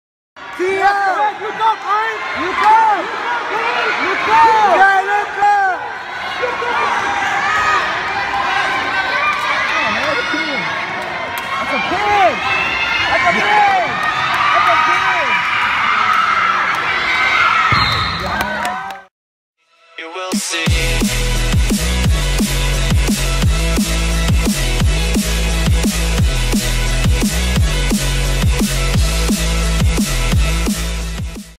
árbitro de lucha hace un raro movimiento y se vuelve viral
Un árbitro de lucha rompe las leyes de la fisica